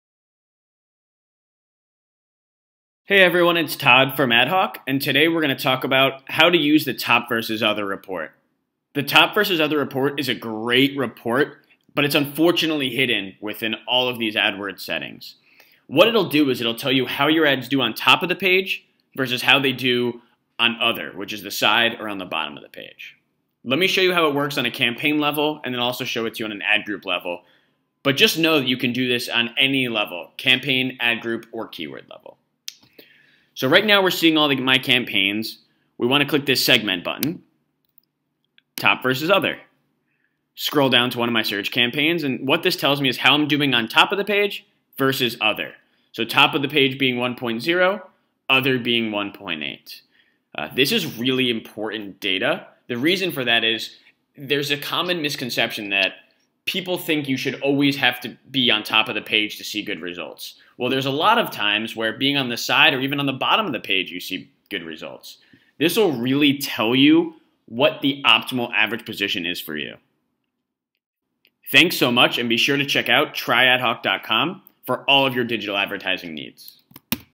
The Top v. Other Report - A Google AdWords Walkthrough
A Google AdWords tutorial to use the Top vs Other report.. Learn how to use the Google AdWords Top vs Other report tool. and our blog if you want more tutorials related to digital marketing, PPC, Facebook ads, Google AdWords or Instagram ads.